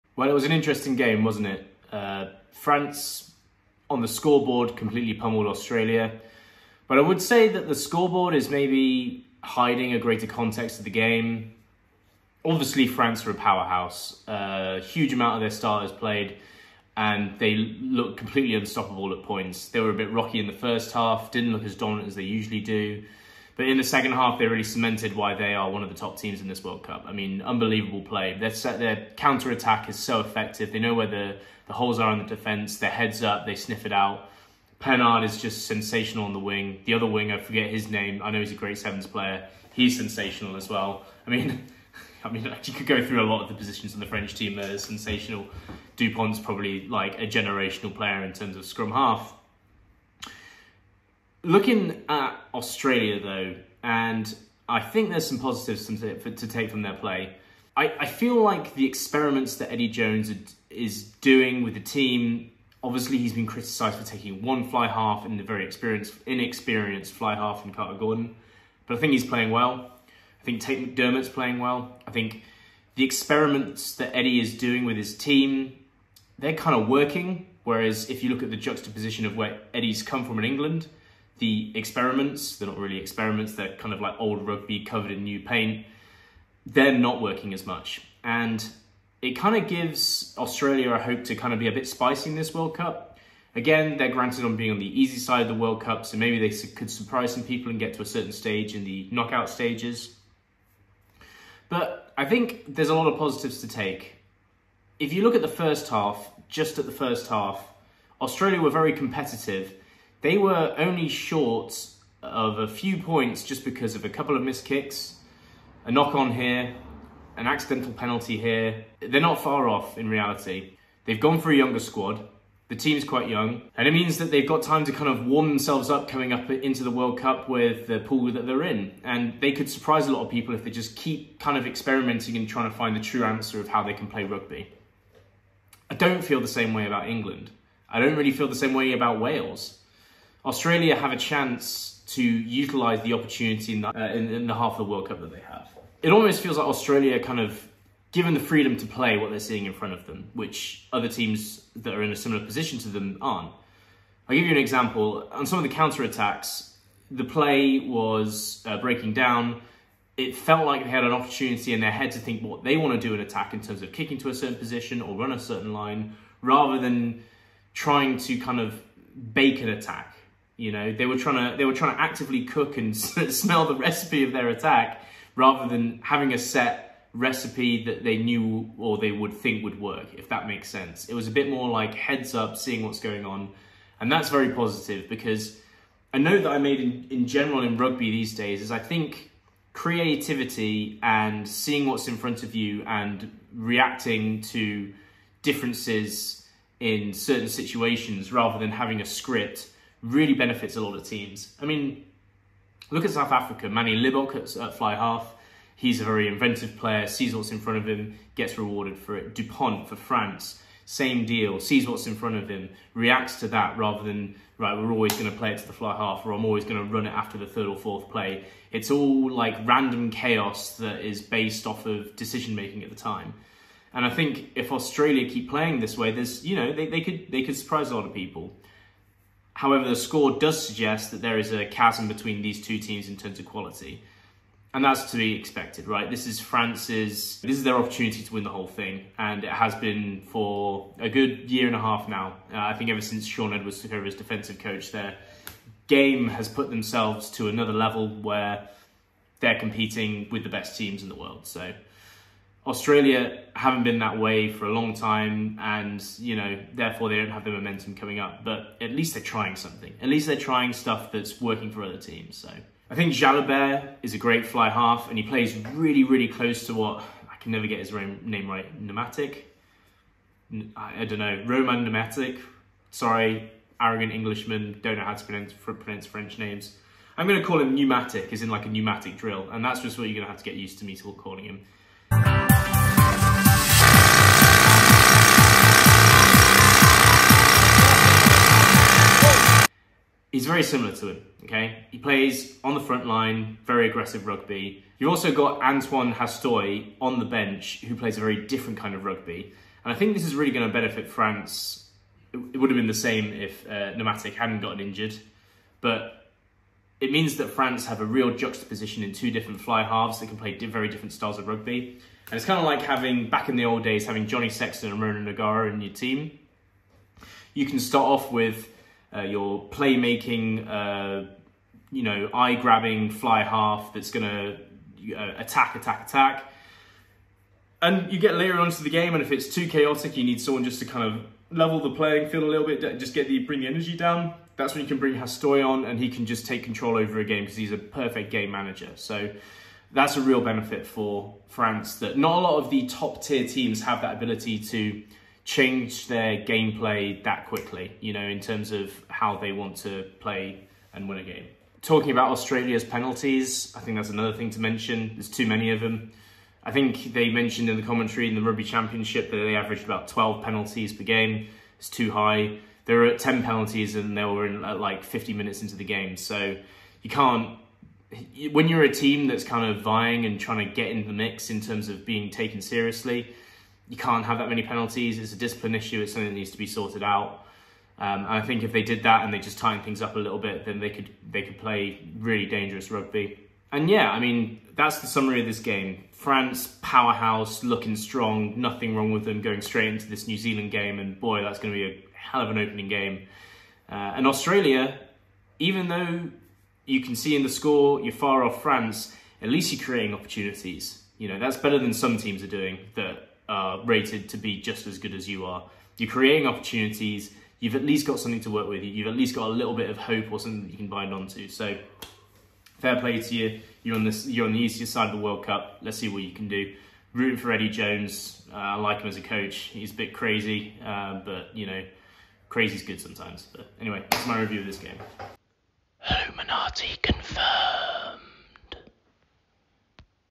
France V Australia | RWC Week 0 Review | FRENCH POWER OVERWHELMS YOUNG WALLABIES
So, not many surprises in this Summer Nations matchup between France and Australia, but something interesting things to note. Australia are at least trying to experiment with how they play, and are given the freedom to read what's in front of them and react to what they see. Considering they are on the "easy" side of the Rugby World Cup Pool's, they could surprise some Rugby fans.

On the French side of things, they look like a true powerhouse moving into the group stages, and their first matchup against the All Blacks is looking particularly interesting. Teams from this weekends game:

France: 15 Thomas Ramos, 14 Damian Penaud, 13 Gael Fickou, 12 Jonathan Danty, 11 Gabin Villiere, 10 Matthieu Jalibert, 9 Antoine Dupont (captain), 8 Gregory Alldritt, 7 Charles Ollivon, 6 Francois Cros, 5 Paul Willemse, 4 Thibaud Flament, 3 Uini Atonio, 2 Julien Marchand, 1 Jean-Baptiste Gros.
Replacements: 16 Peato Mauvaka, 17 Sebastien Taofifenua, 18 Dorian Aldegheri, 19 Romain Taofifenua, 20 Cameron Woki, 21 Paul Boudehent, 22 Baptiste Couilloud, 23 Melvyn Jaminet.

Australia: 15 Andrew Kellaway, 14 Mark Nawaqanitawase, 13 Jordan Petaia, 12 Lalakai Foketi, 11 Suliasi Vunivalu, 10 Carter Gordon, 9 Tate McDermott, 8 Rob Valetini, 7 Fraser McReight, 6 Tom Hooper, 5 Will Skelton, 4 Richie Arnold, 3 Taniela Tupou, 2 David Porecki, 1 Angus Bell.
Replacements: 16 Matt Faessler 17 Blake Schoupp, 18 Zane Nonggorr, 19 Matt Philip, 20 Rob Leota, 21 Langi Gleeson, 22 Issak Fines-Leleiwasa, 23 Ben Donaldson